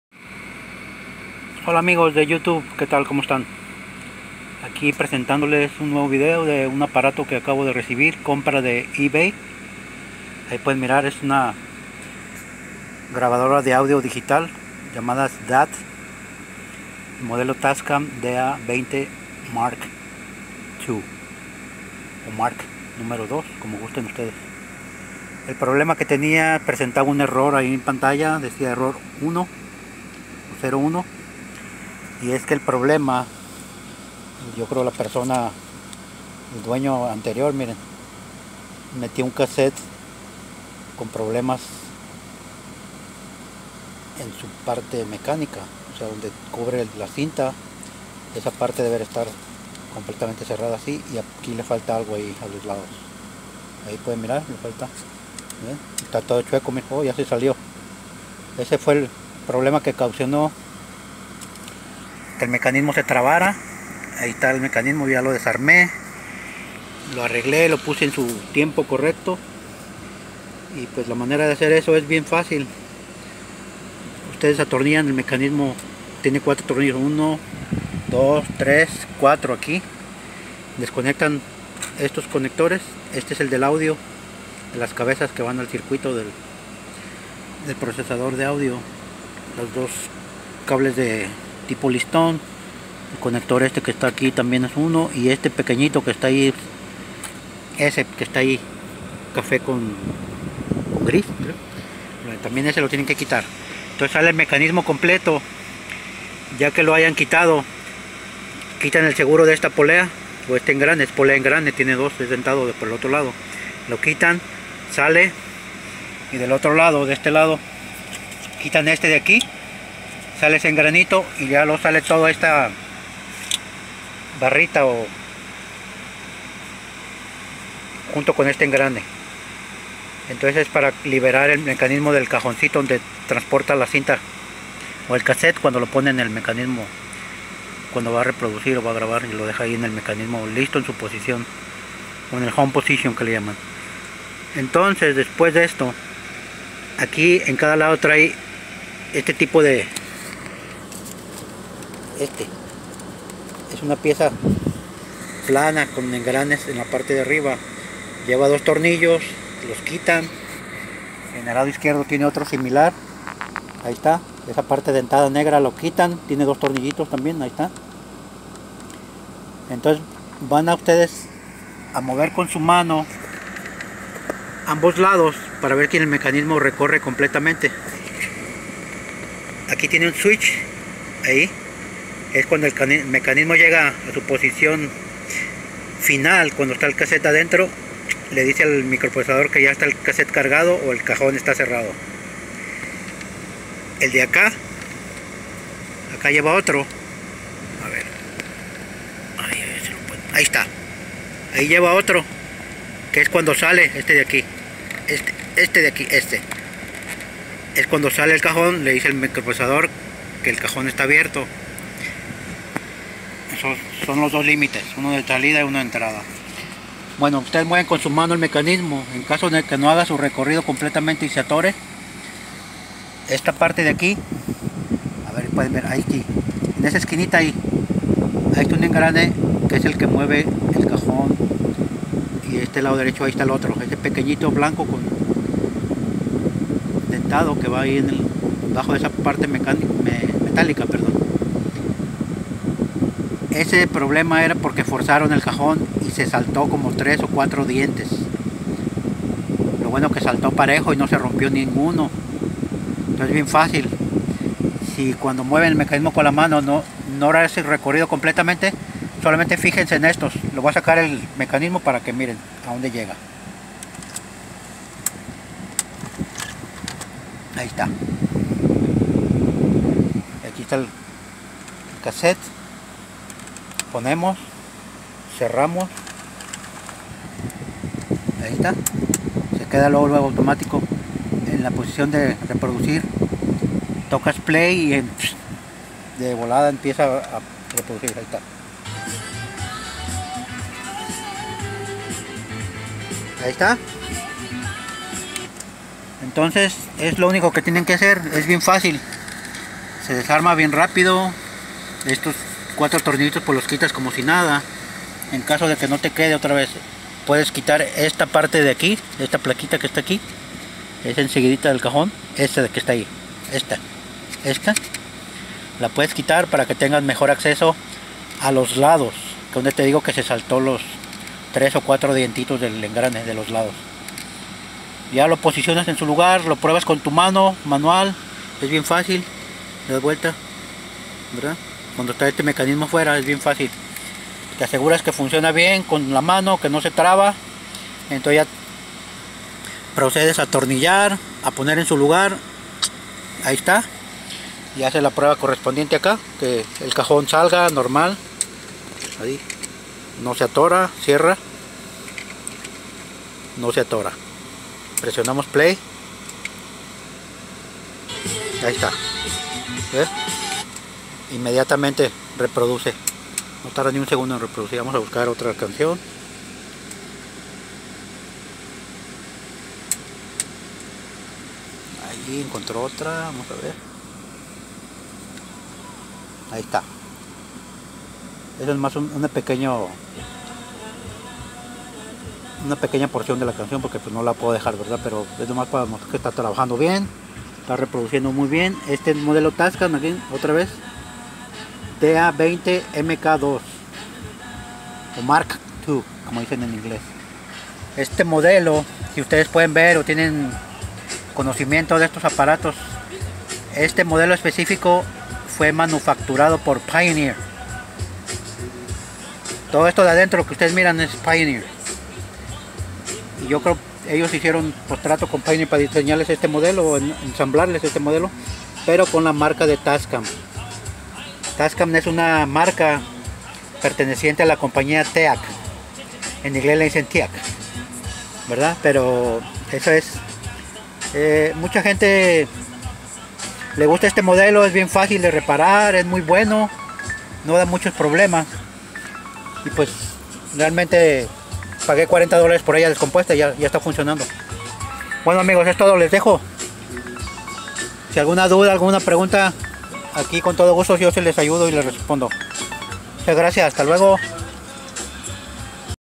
TASCAM DA-20 MKII Error 01 Mecanismo Forzado/No sale el Cassette.
En esta ocasión les presento esta grabadora digital DAT . Que presentaba error 01 en pantalla y no lograba expulsar el cajón/Cassette.